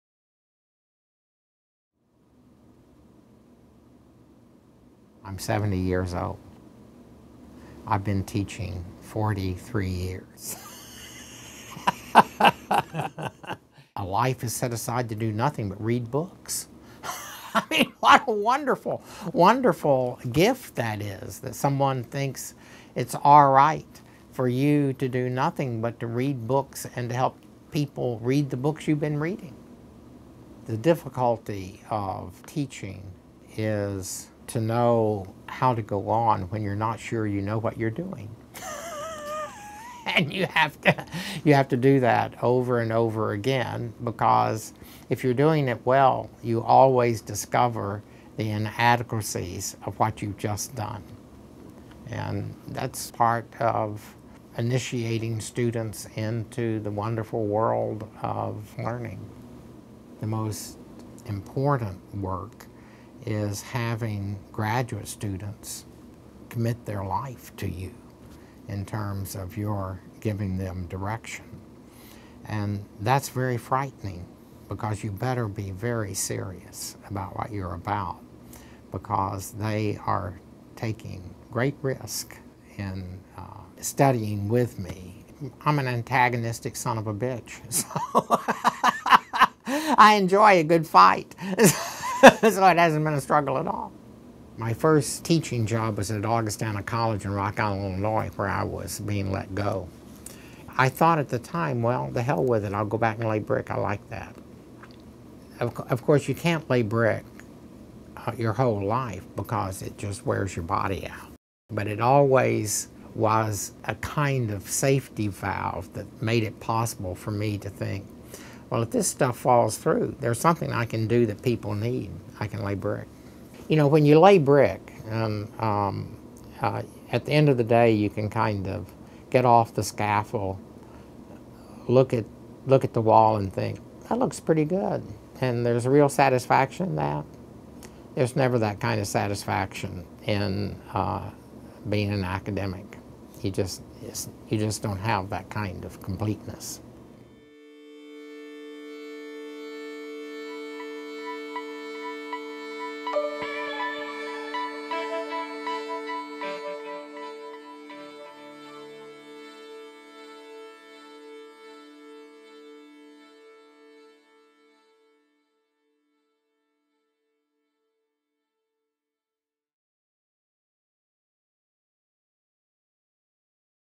Stanley Hauerwas, Duke Divinity School's Bricklaying Theologian
Stanley Hauerwas, Professor of Theological Ethics, Duke Divinity School, on bricklaying, teaching and the satisfaction of completing one's work. Stanley is the author of numerous books, including the recent memoir, "Hannah's Child: A Theologian's Memoir."  In 2001 Time magazine named "America's Best Theologian." Stanley responded by saying, "'Best' is not a theological category."